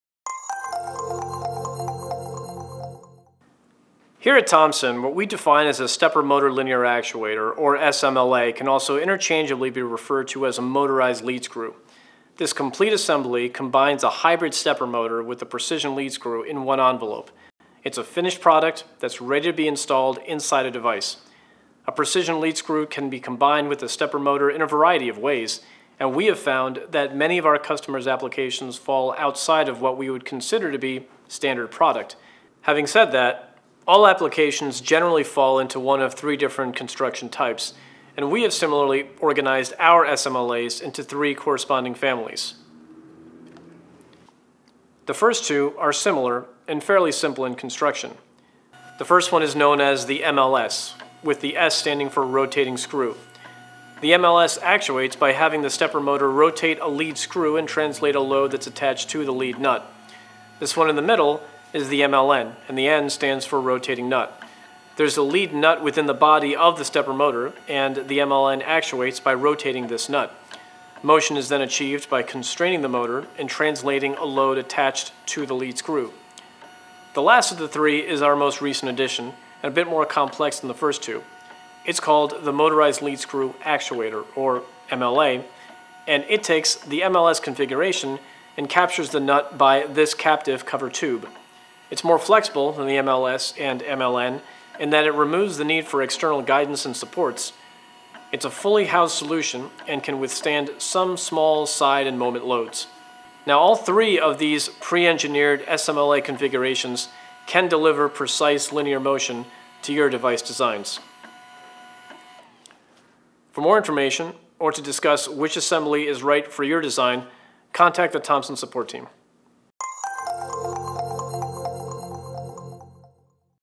Tech Tips: Stepper Motor Linear Actuator Assembly Configurations
Precision lead screws can be combined with a stepper motor in a number of ways. At Thomson, we offer three configurations of stepper motor linear actuators to meet the various needs of our customers' applications. Learn more about them and discover which of them can benefit your linear motion designs.